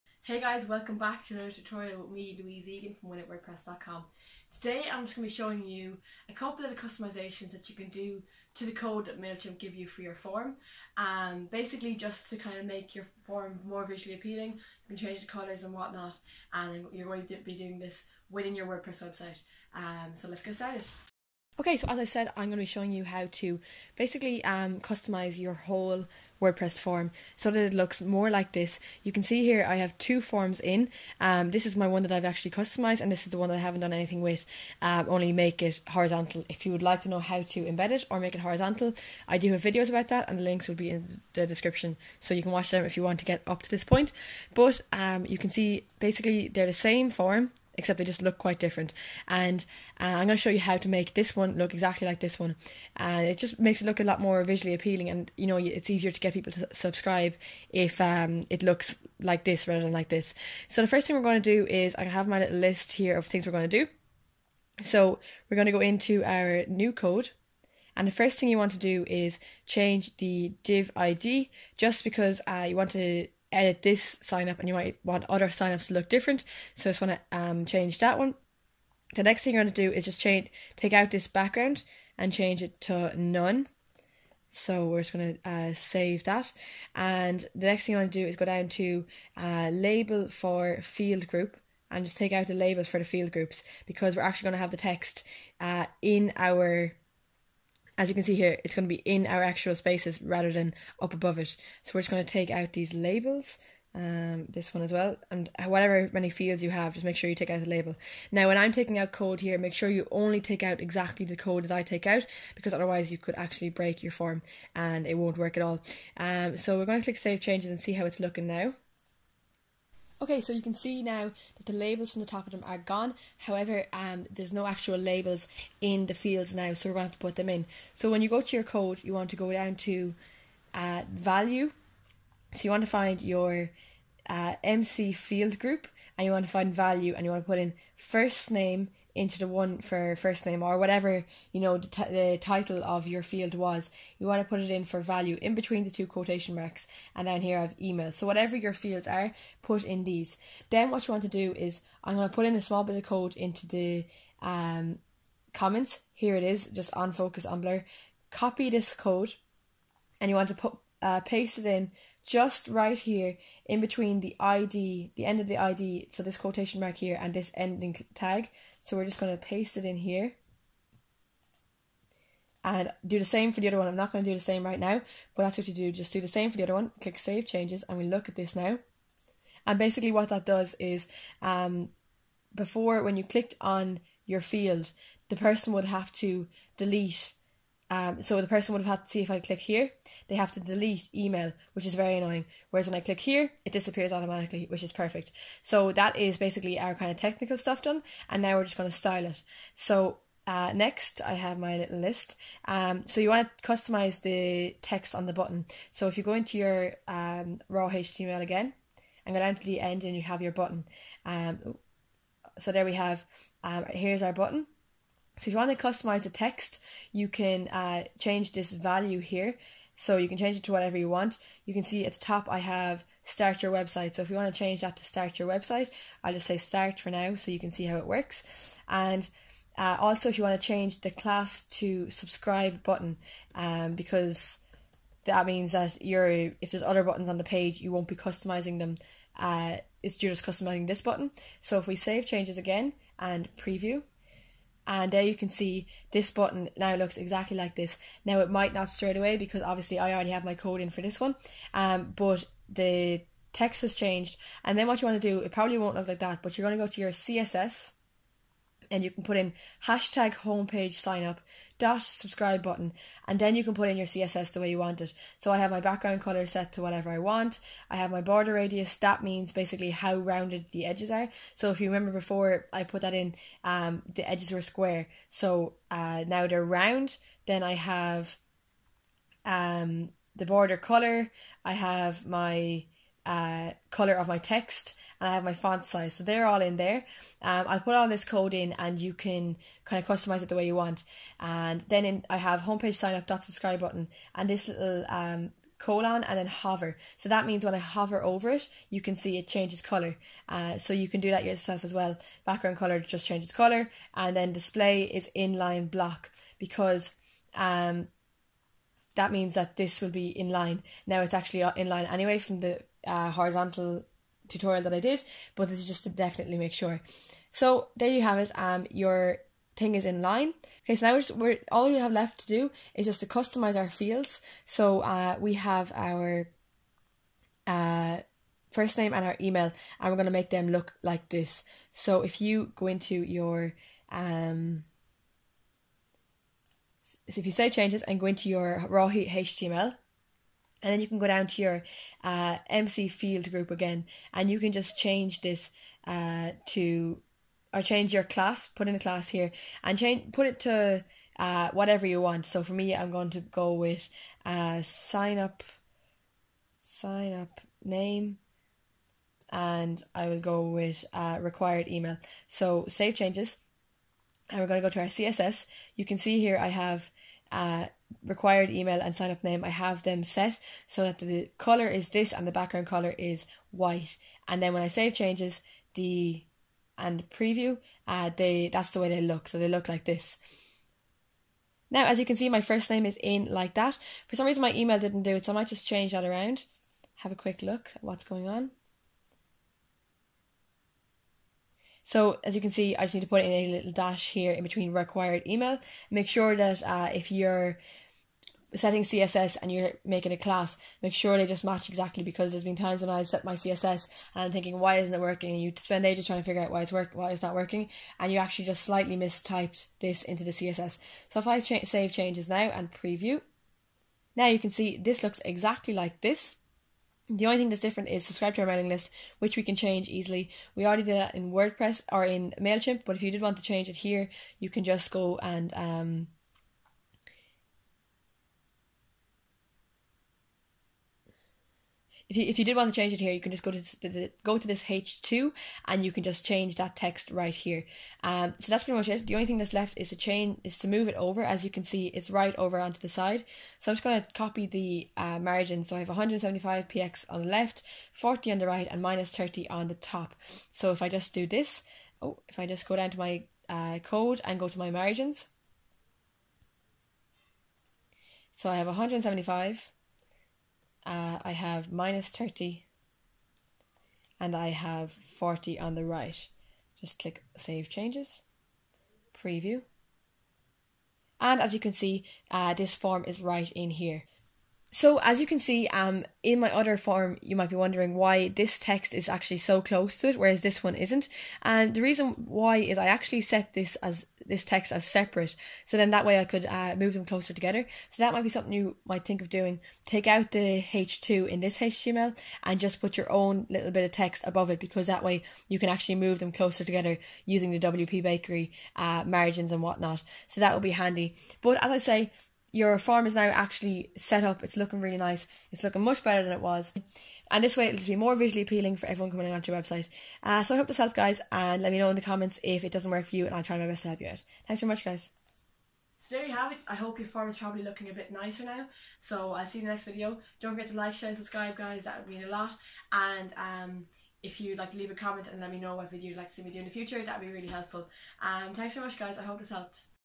How to Customise Your Mailchimp Embedded Form - Within Wordpress
Learn how to customise your Mailchimp embedded form using a small bit of CSS in order to get it to look exactly the way you'd like!

Code Used:
1. Change Div Id to homepage-signup

2. Change background to none

3.Take out label tags

4. Change value of field groups

5. Add code to make words disappear (Add to each field):
   onfocus="if(this.value==this.defaultValue)this.value='';"
   onblur="if(this.value=='')this.value=this.defaultValue;"

7. Change classes of fields to signupname and required-email

8. Custom CSS Added:
Customise Button:
background-color: 048dcb;
    border-radius: 5px;
border-color: 048dcb;
color: fff;
    font-size: 16px;

background-color: 0059a2;
color: fff;
    display: inline-block;

Customise Fields:
color: 048dcb;
background-color: fff;

color : 048dcb;
background-color: fff;

9. Add in margins to center form